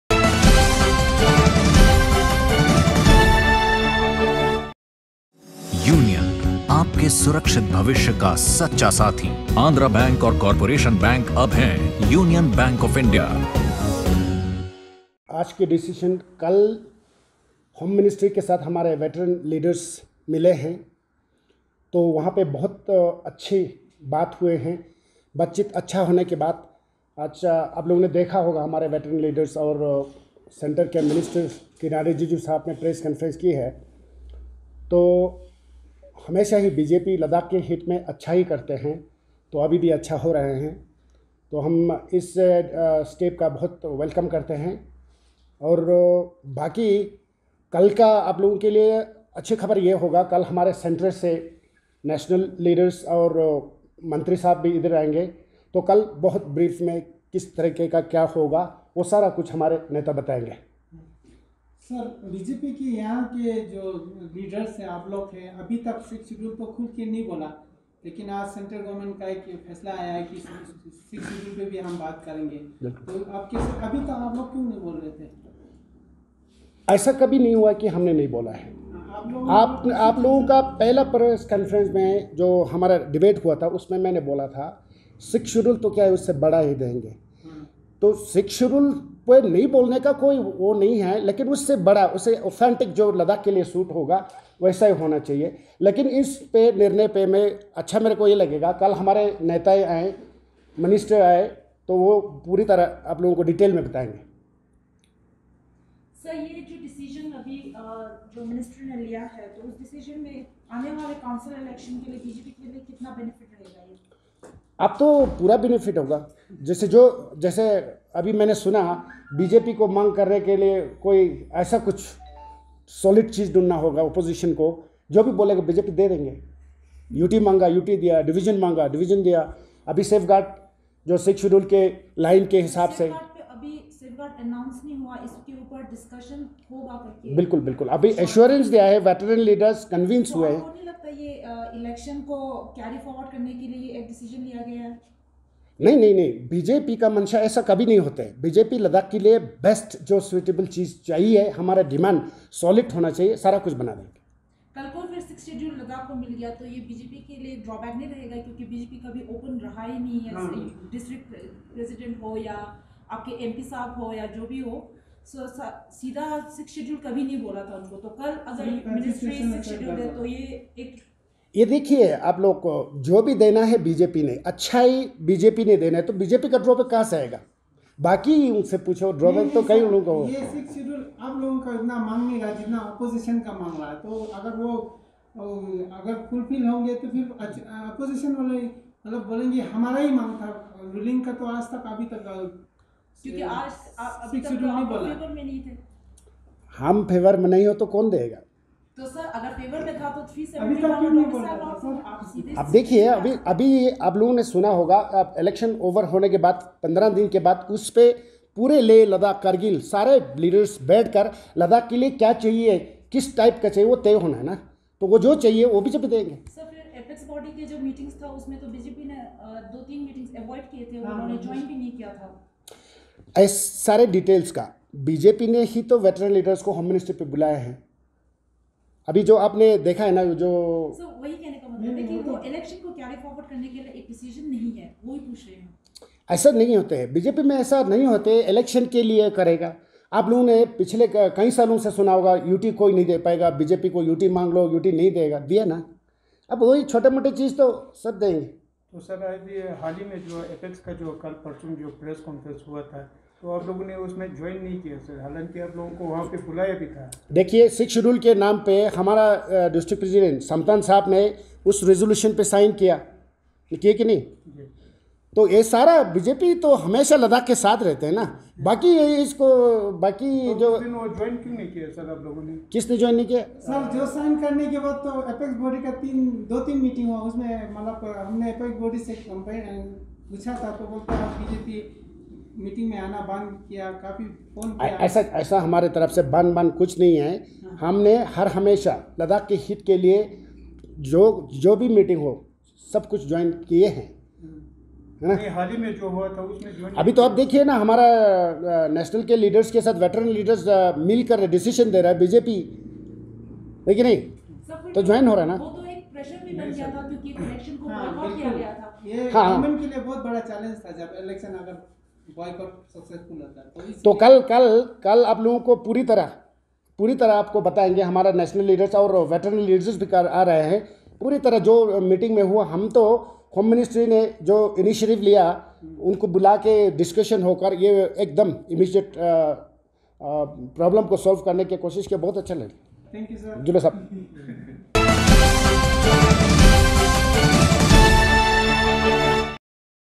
Dorje Angchuk, BJP Ladakh holds conference on various issues of Ladakh - Focus News
Dorje Angchuk, BJP Ladakh holds conference on various issues of Ladakh including Veterans Leaders of Ladakh meeting Home Minister Amit Shah - Focus News